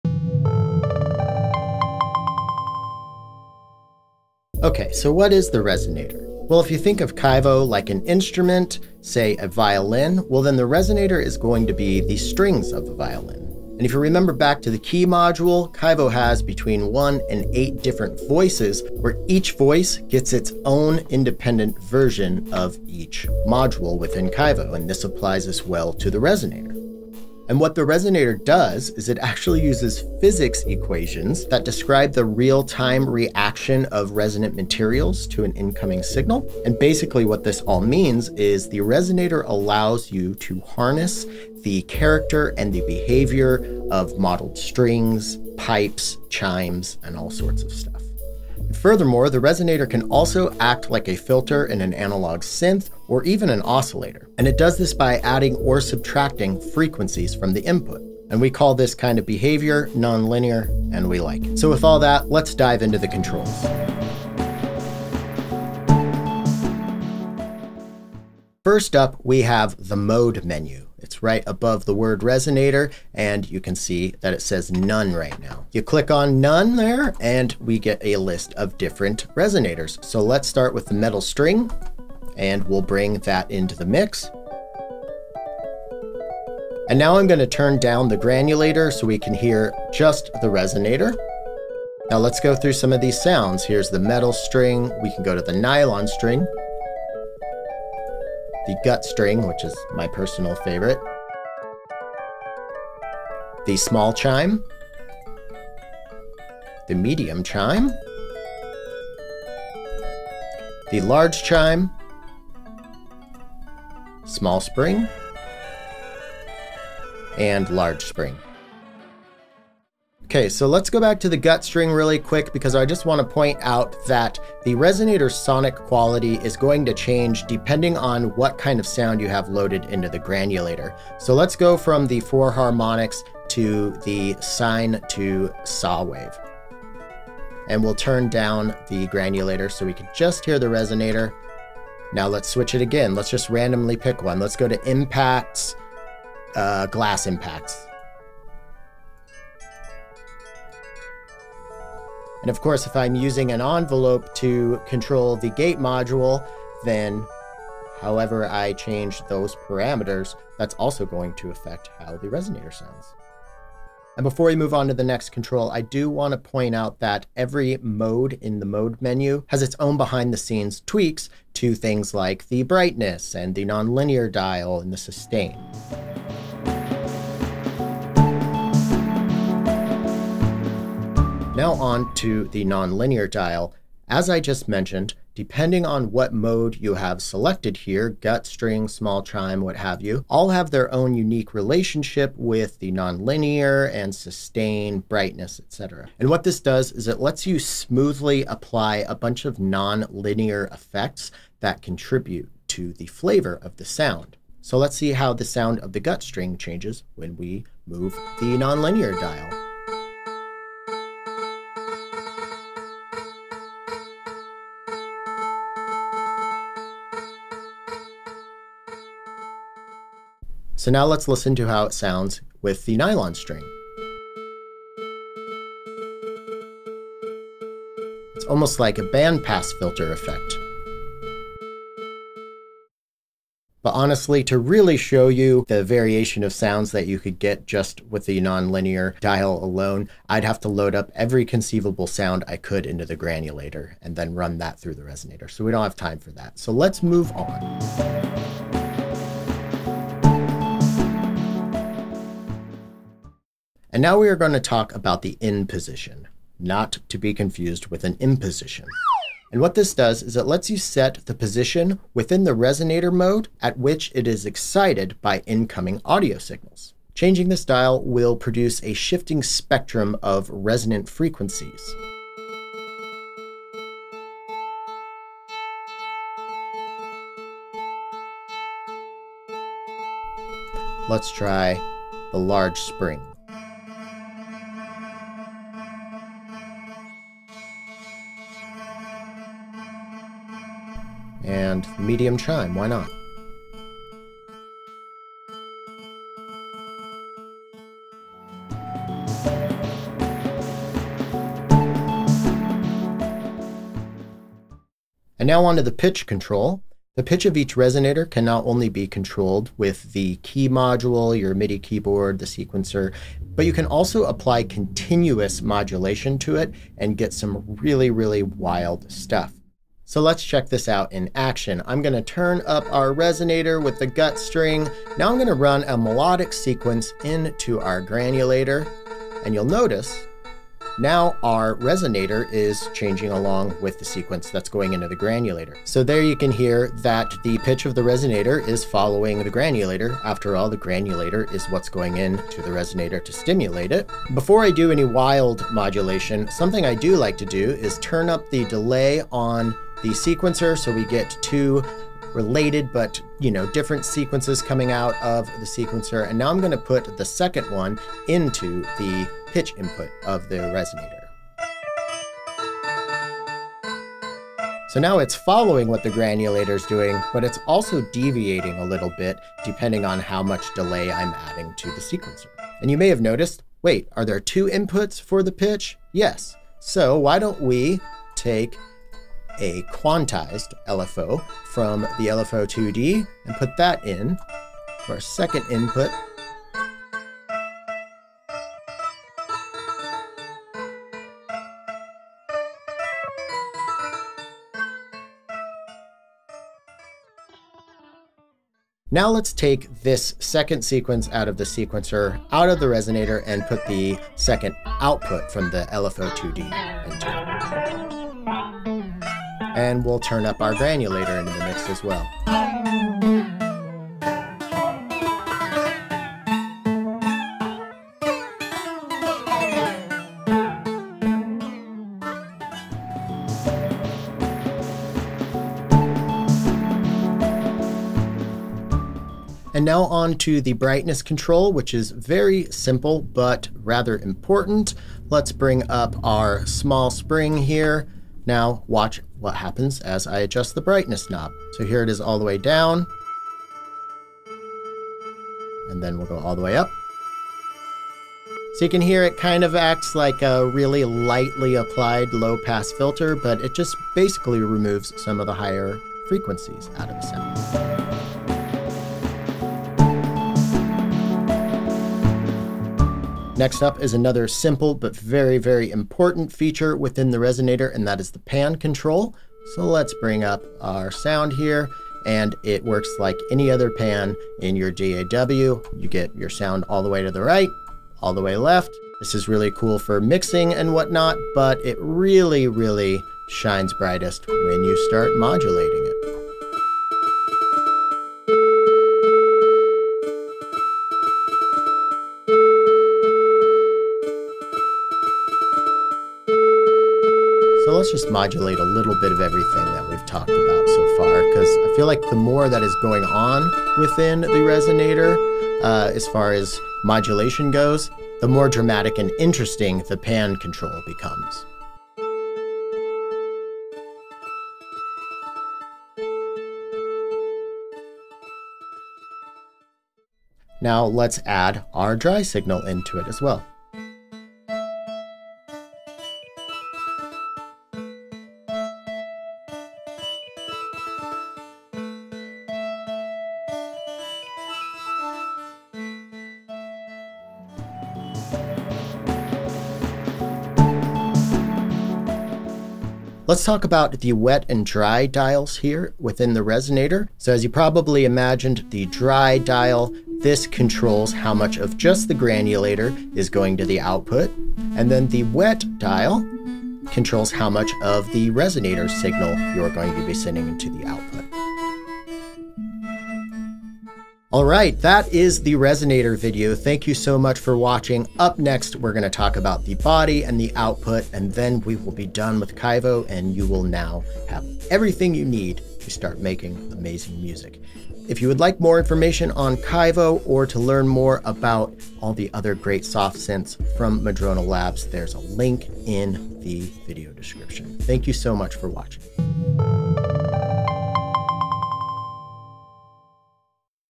Kaivo: Resonator Overview
Overview of Kaivo's Resonator Module

Kaivo is a software instrument combining two powerful synthesis techniques (physical modeling and granular synthesis) in an easy-to-use semi-modular package. It’s laid out a bit like an acoustic instrument; the GRANULATOR module acts like the player’s touch, exciting one or more tuned objects (here, the RESONATOR module, based on physical models of resonant objects) that come together in a central resonating body (the BODY module, also physics-based).

CHAPTERS
00:00 Intro
01:04 Mode Menu
02:57 Nonlinear Dial
04:11 In Position
05:00 Pitch Control
07:03 Brightness Control
07:38 Pan Control
09:01 Wet/Dry Control